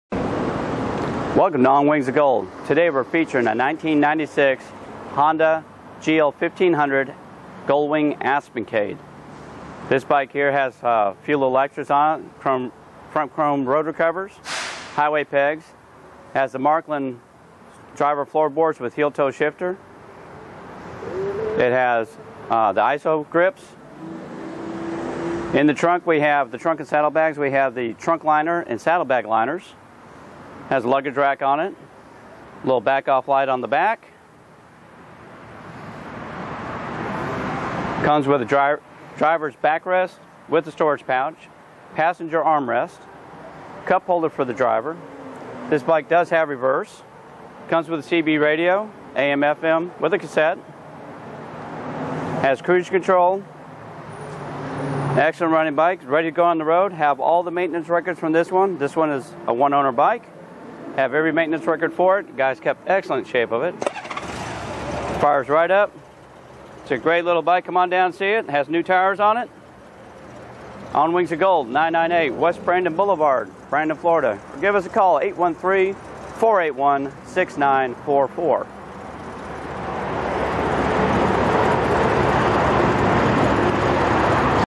1996 Honda Goldwing Aspencade
This is a beautiful 1996 Honda Gold Wing Aspencade Blue, with lots of extras, has bag liners and tool kit